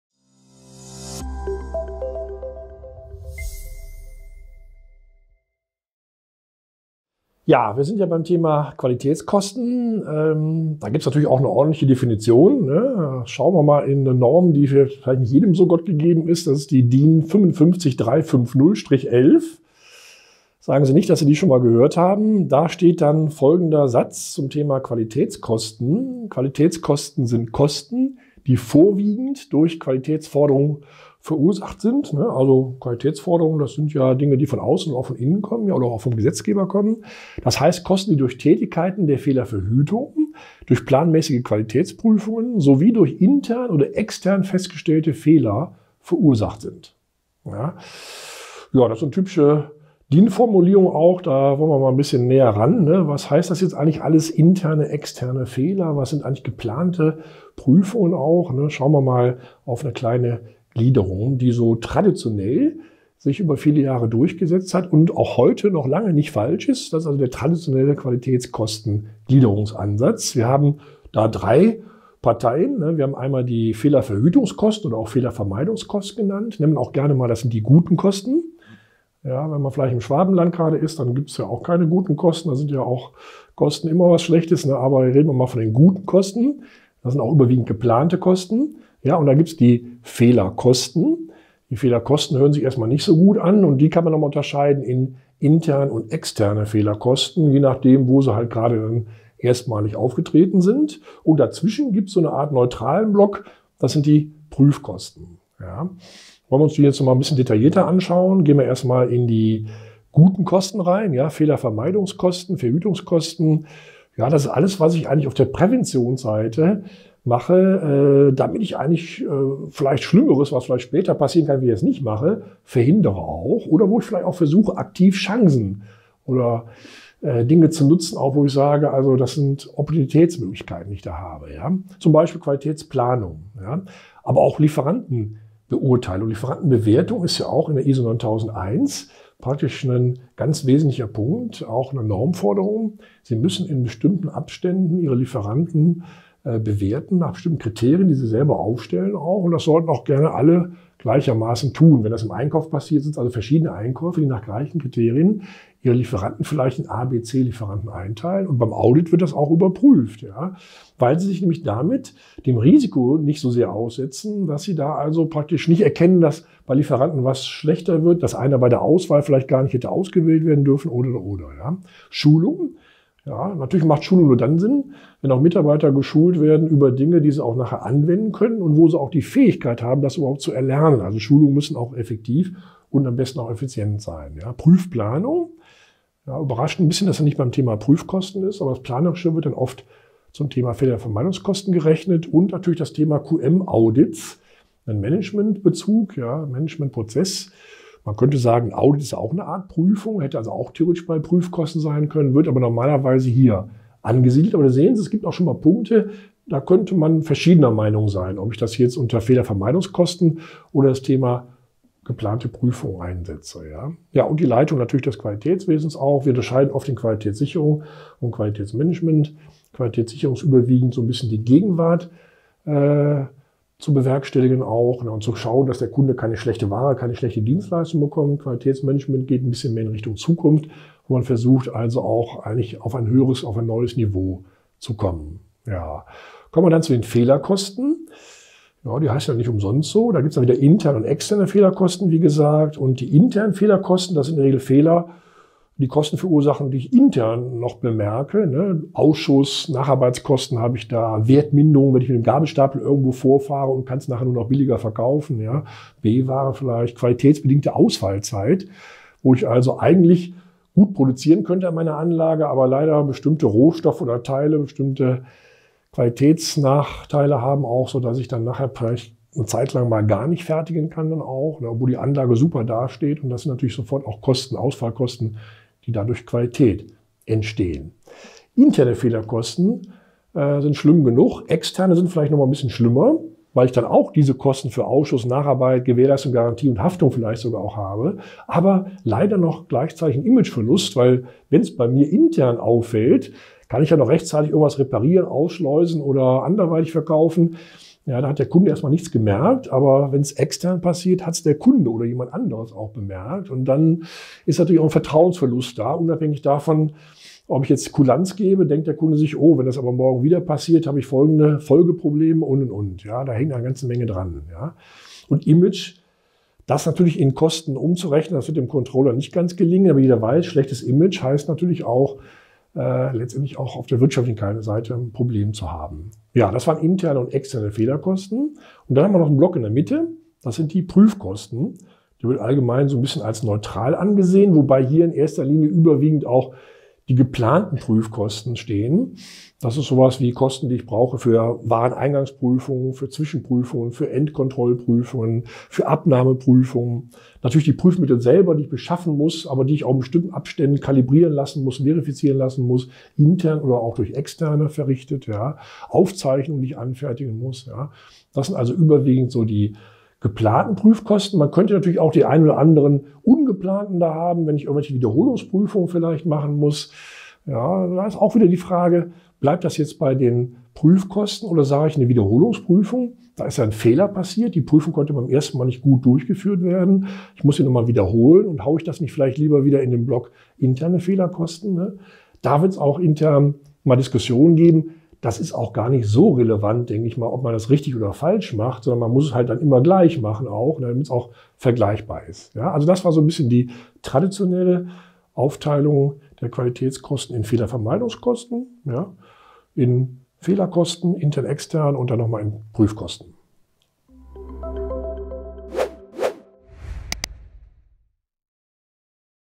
Was sind Qualitätskosten und wie sind diese gegliedert?
In diesem Video geht es um die Qualitätskosten sowie die Qualitätskostengliederung. Unter Qualitätskosten versteht man Kosten, die durch die Fehlerverhütung (Qualitätsforderungen) sowie zur Sicherstellung der Produkt- und Dienstleistungsqualität zu Stande kommen. Die Gliederung der Qualitätskosten zeigen wir Ihnen in diesem Video anhand von Beispielen.

Inhalte dieses Videos:
0:00 Wie werden Qualitätskosten definiert?
0:59 Gliederung der Qualitätskosten (Grafik)
1:46 Was sind Fehlerverhütungskosten?
3:56 Was sind interne und externe Fehlerkosten?
5:29 Wovon sind Prüfkosten abhängig?